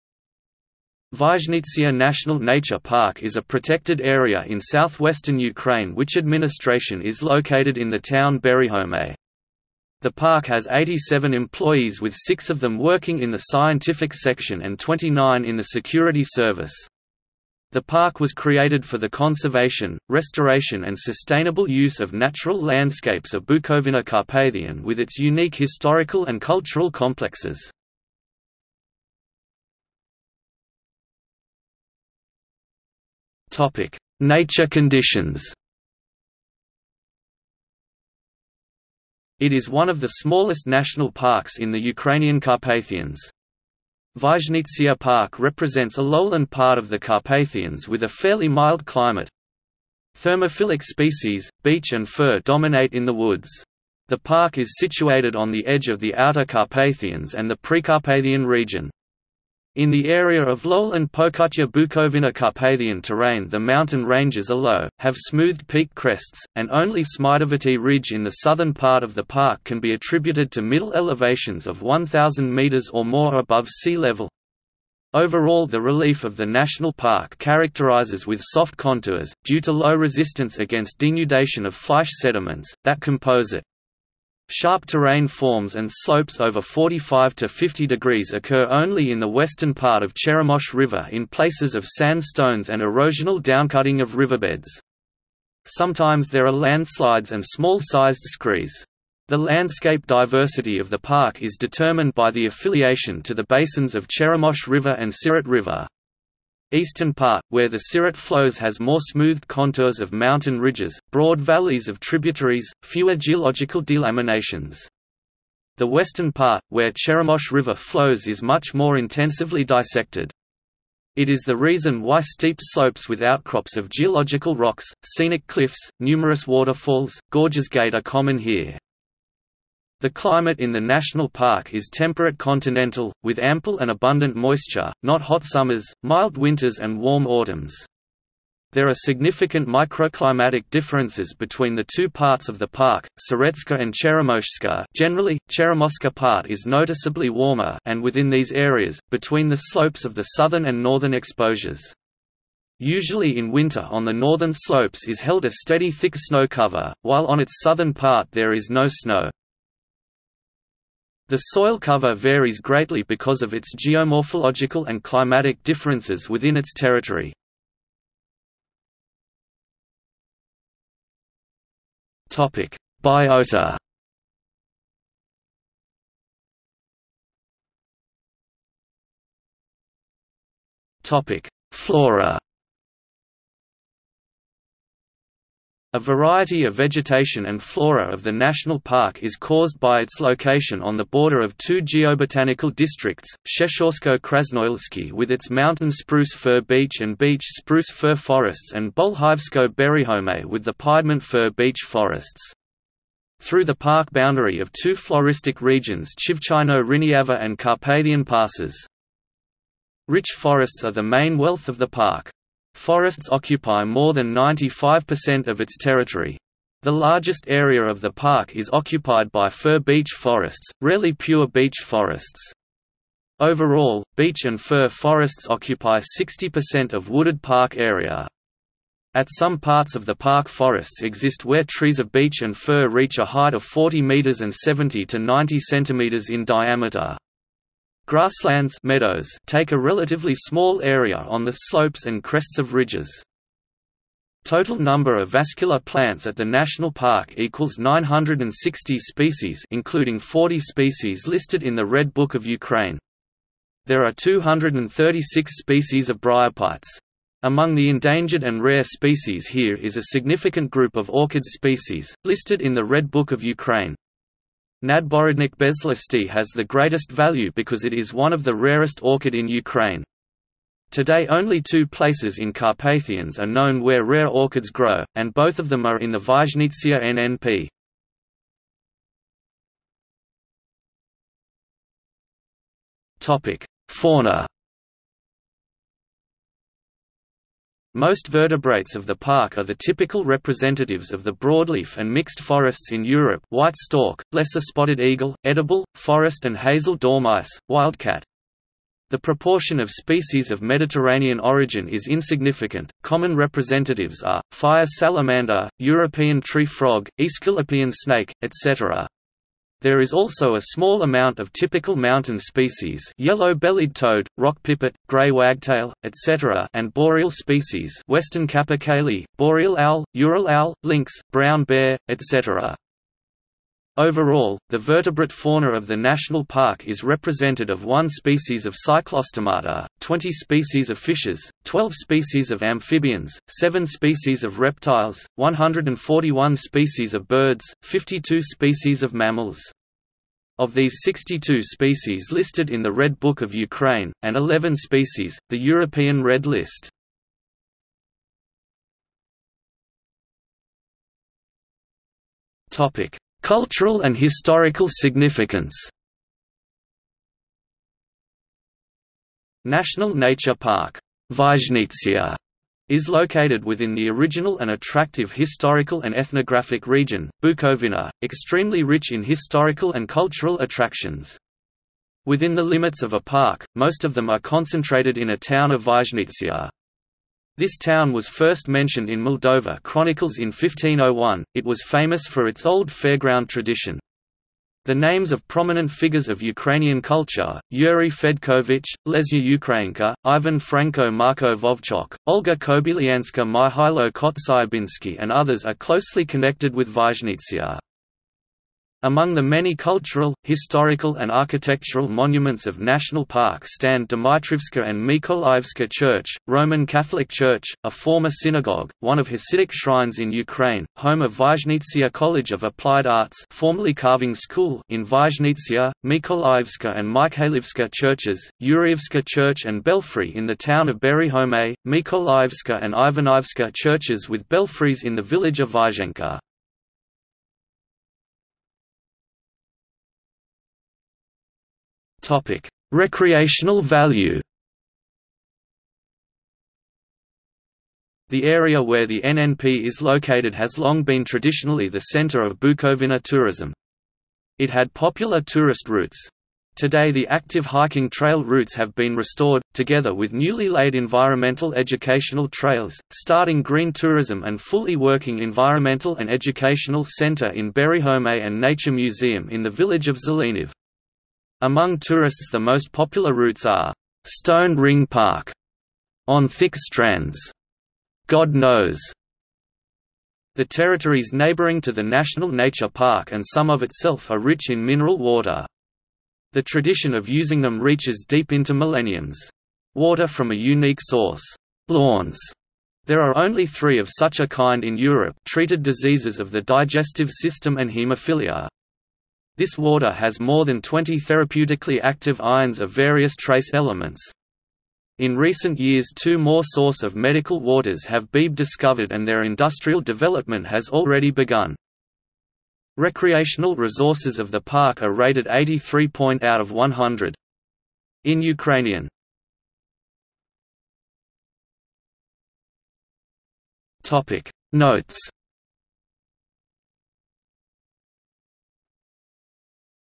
Vyzhnytsia National Nature Park | Wikipedia audio article
00:00:29 1 Nature conditions
00:02:48 2 Biota
00:02:57 2.1 Flora
00:04:39 2.2 Fauna
00:05:48 3 Cultural and historical significance
00:07:08 4 Recreational value
00:08:30 5 Notes

- Socrates

SUMMARY
Vyzhnytsia National Nature Park is a protected area in south-western Ukraine which administration is located in the town Berehomet. The park has 87 employees with 6 of them working in the scientific section and 29 in the security service. The park was created for the conservation, restoration and sustainable use of natural landscapes of Bukovyna Carpathian with its unique historical and cultural complexes.